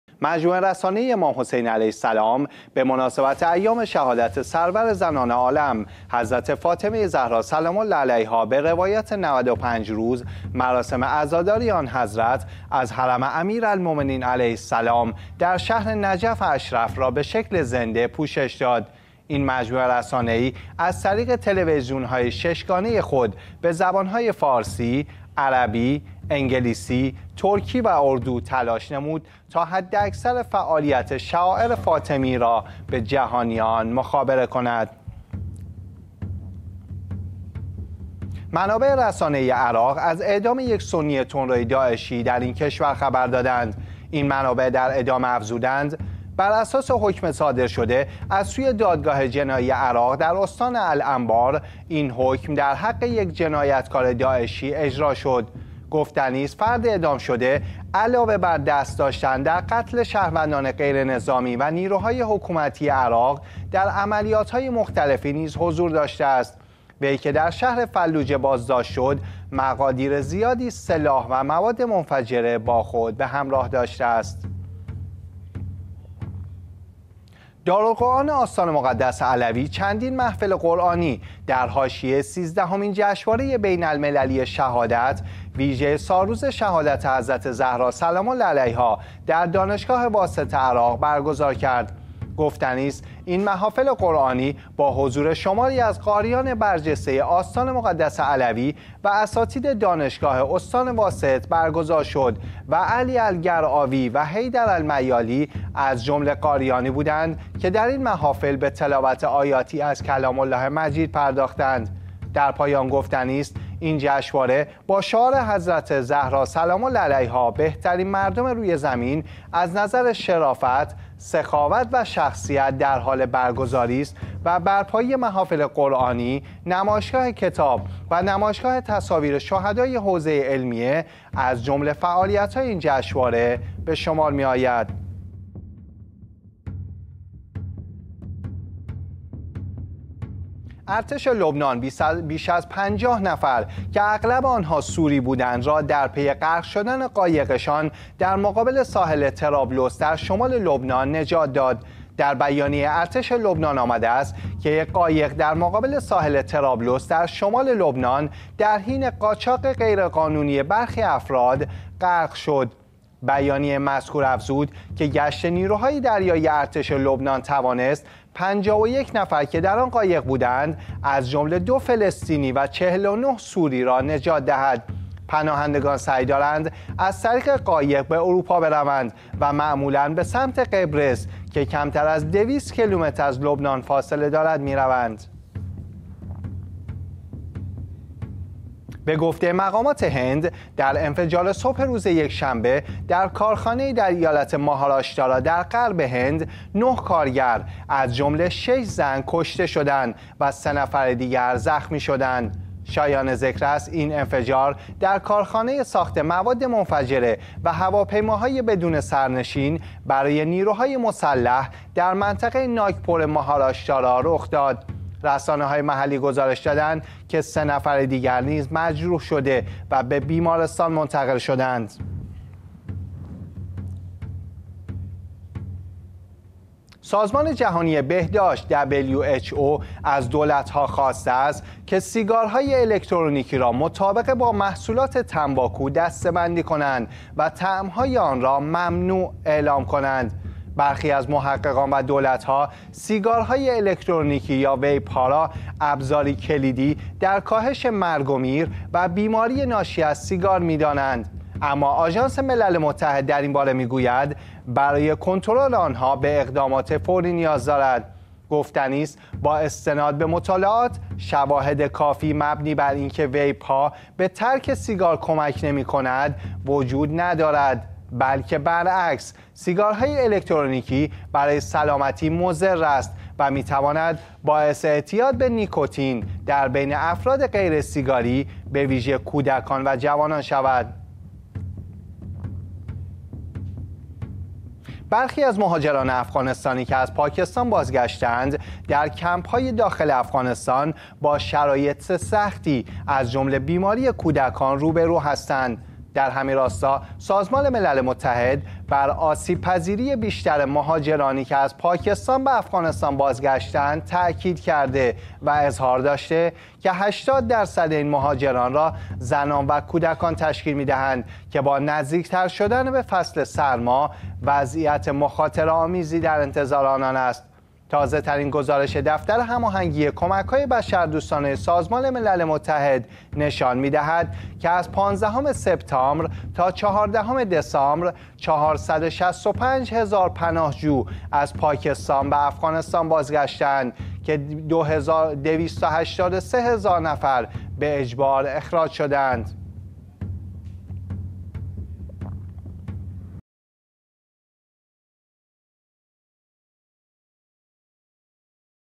خبر پانوراما : دوشنبه 27 آذر ماه 1402
-پوشش زنده مراسم ایام فاطمیه از سوی مجموعه رسانه ای امام حسین علیه السلام
-اعدام یک سنی تندروی د۱عشی در عراق
-برپایی محافل قرآنی در جشنواره بین‌المللی «حضرت زهرا سلام الله علیها»در عراق
-نجات ۵۱ مهاجر در پی غرق شدن قایق‌شان در شمال لبنان
9 کشته و سه زخمی در پی انفجار در کارخانه ای در غرب هند
-درخواست سازمان بهداشت جهانی از دولت‌ها برای اجرای ممنوعیت سیگارهای الکترونیکی
-سازمان ملل: شرایط مهاجران افغانستانی بازگشته از پاکستان وخیم است